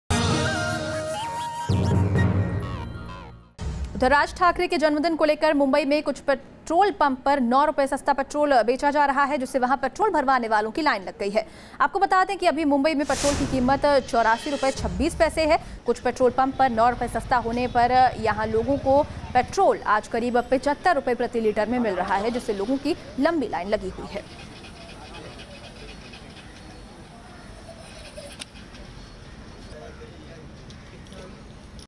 Mumbai: On Raj Thackeray's birthday, some pumps selling petrol at reduced prices
Angered by the hike in petrol and diesel prices for 16 consecutive days and with marginal less for the next 14 days, the Maharashtra Navnirman Sena has decided to give relief to Mumbaikars by offering petrol for Rs 4 less than the market price on the birthday of party president Raj Thackeray